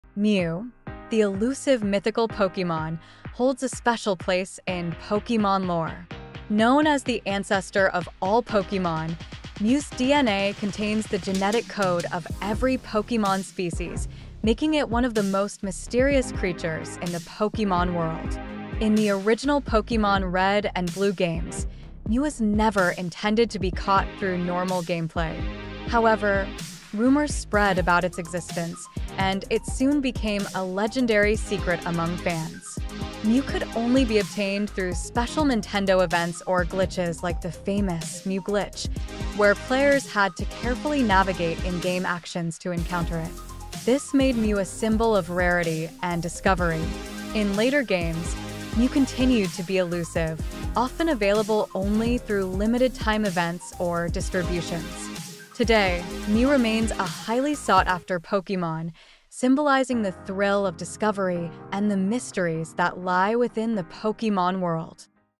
Facts About The Legendary Pokémon Mew
Discover fascinating facts about Mew, the legendary Mythical Pokémon known as the ancestor of all Pokémon! Learn about its origins, abilities, and why it’s considered one of the world's rarest and most mysterious Pokémon. Dive into the lore behind this elusive psychic type and uncover the secrets that make Mew a true icon in the Pokémon universe!